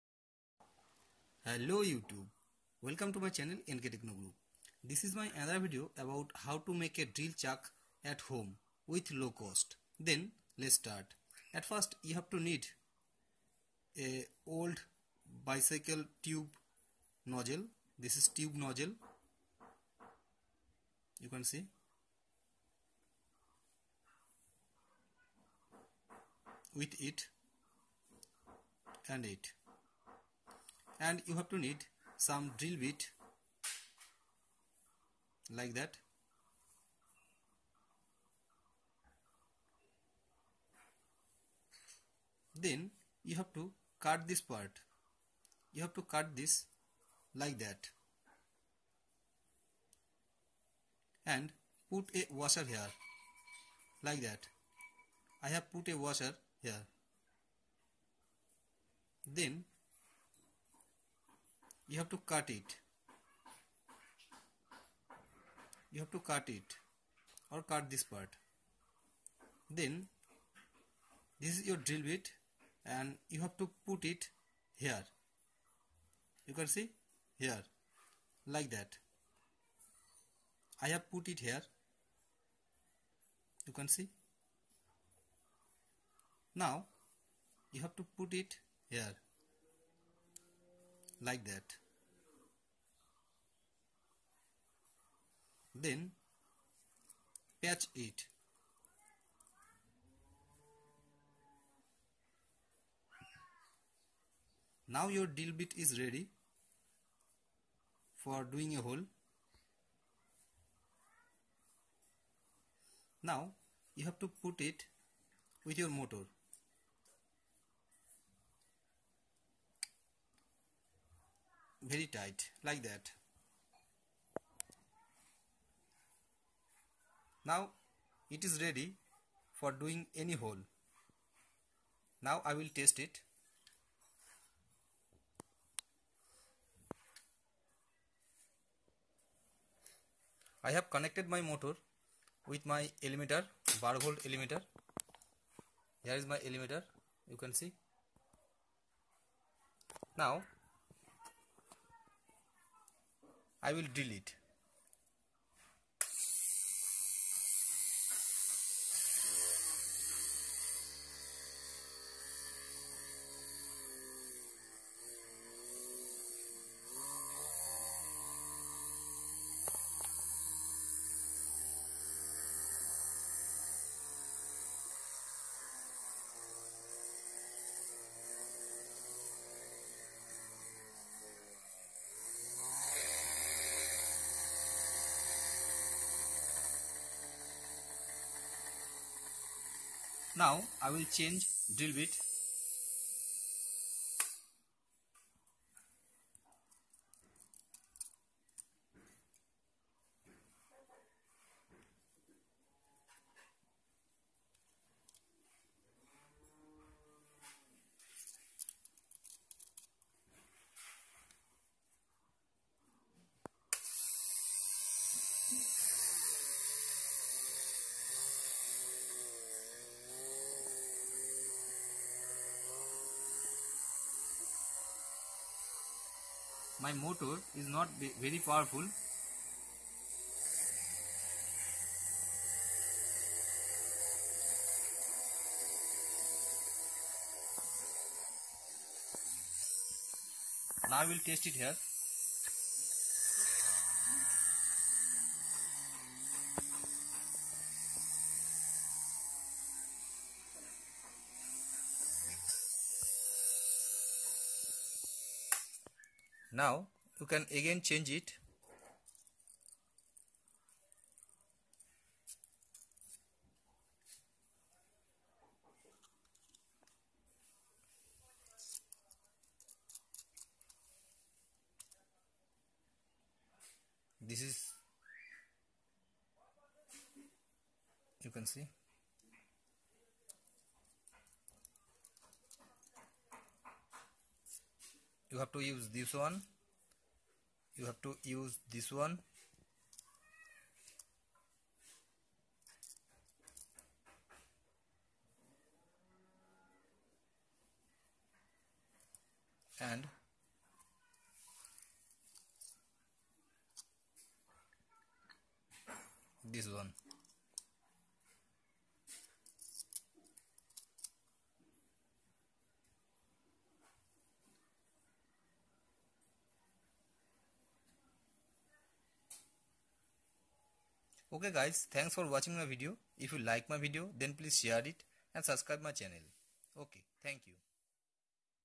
HOW TO MAKE DRILL CHUCK AT HOME VERY LOW COST
this video about HOW TO MAKE DRILL CHUCK AT HOME WITH LOW COST. IF YOU DO NOT WANT TO BUY DRILL CHUCK THEN THIS IDEA WILL HELP YOU TO MAKE IT AT HOME WITH LOW COST.